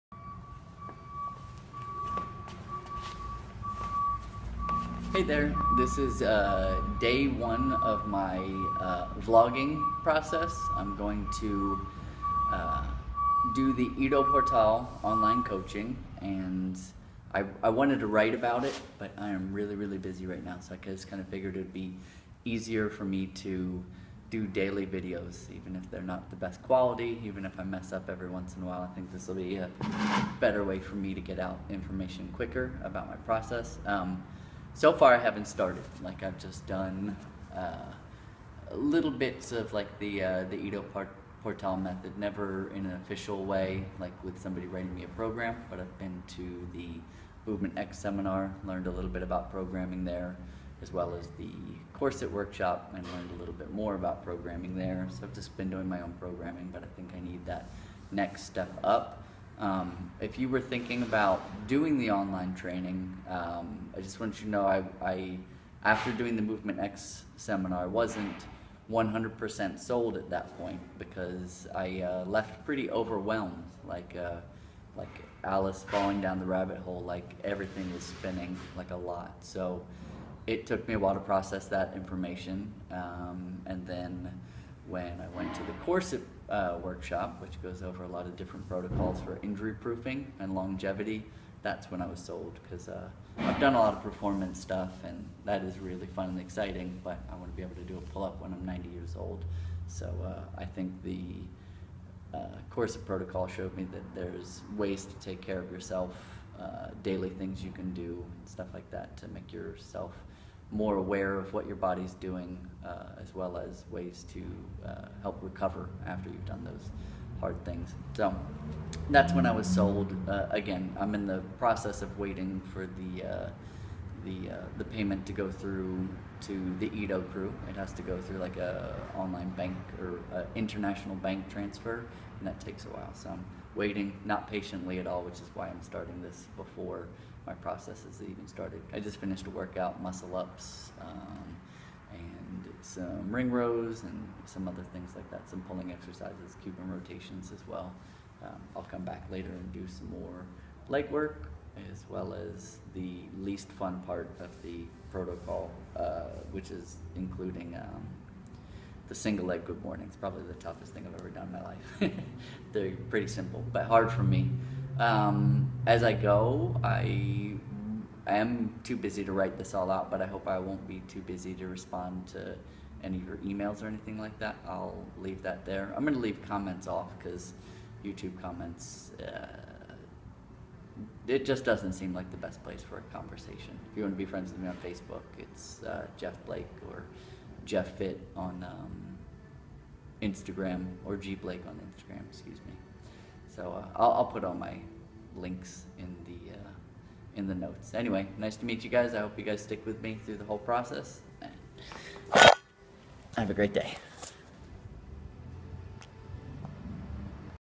Vlog #1 Ido Portal Online Coaching
This is where I'm going to document my journey doing the Ido Portal Online Coaching.

I'll be talking about my experience, but I won't be sharing my program. I find value in their coaching so I pay for it. If you find value in being coached, contact them or find a coach/teacher in your area.

I'm 36 years old. I volunteer at a non-profit gym in Baltimore city, , and that's where I'm a coach/personal trainer as well. More isn't better, better is better.